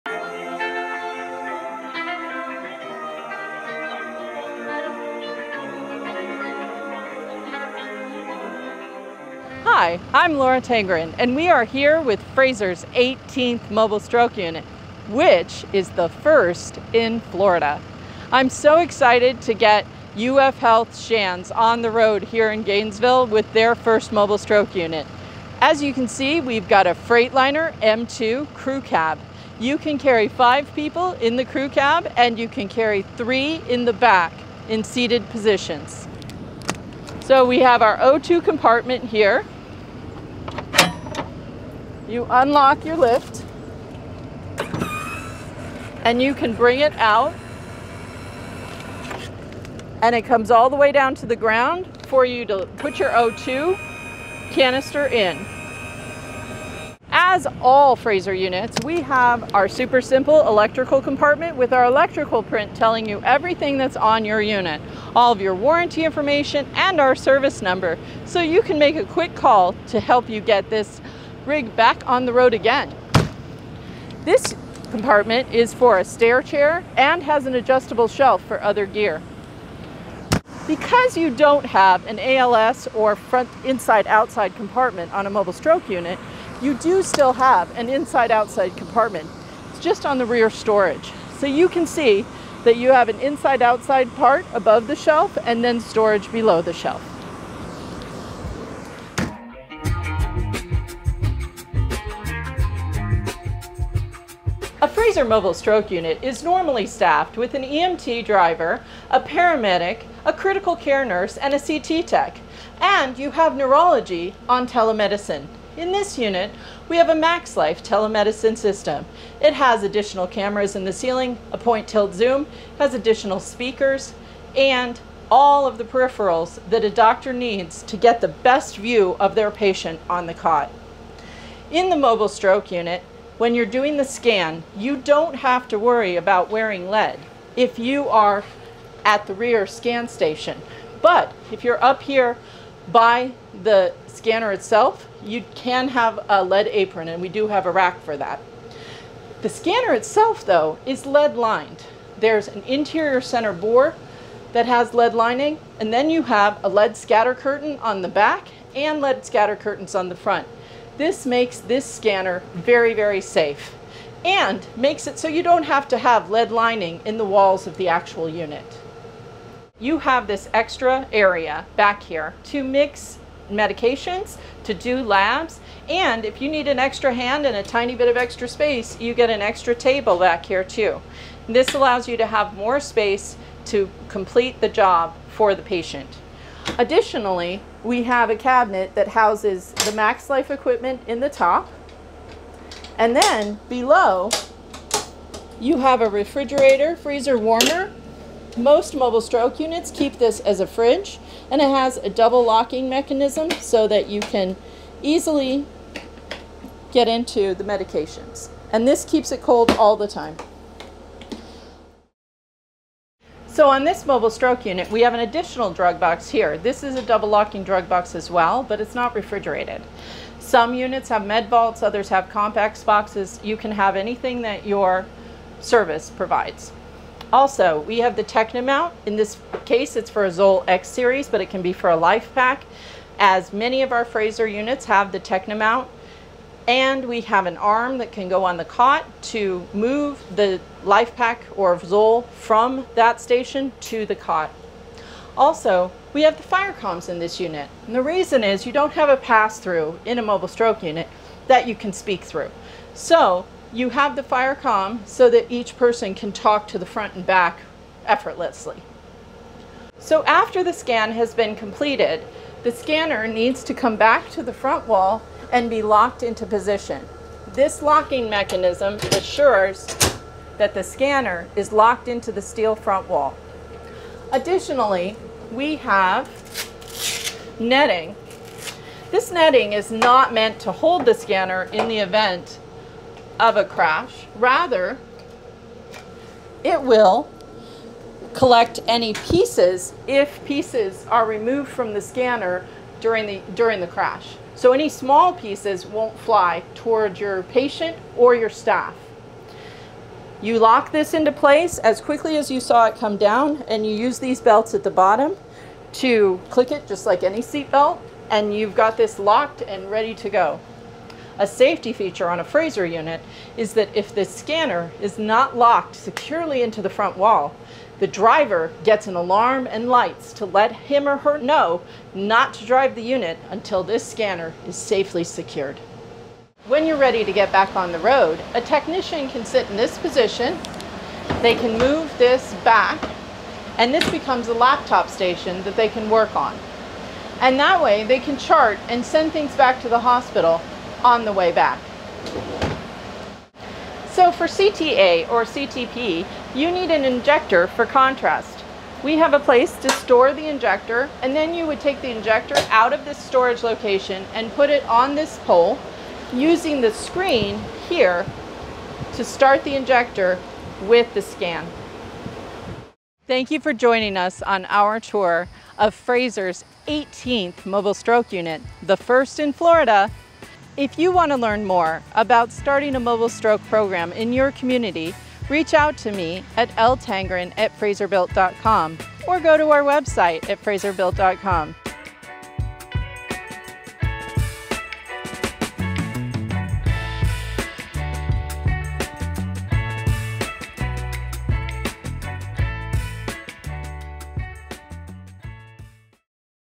Take a Tour of the NEW UF Health Mobile Stroke Unit!
Get a full tour of the brand new UF Health Mobile Stroke Unit!

Laura introduces the brand new mobile stroke unit Frazer build. Tour the exterior compartments, crew cab, interior workspace, and CT scanner area.

Learn about this brand new technology in mobile stroke care and be sure to also watch the scenes from the exciting kick-off event for UF Health!

To get more information about these unique custom units, visit the Mobile Stroke Unit page at frazerbilt.com.

Defining the future of Mobile Healthcare.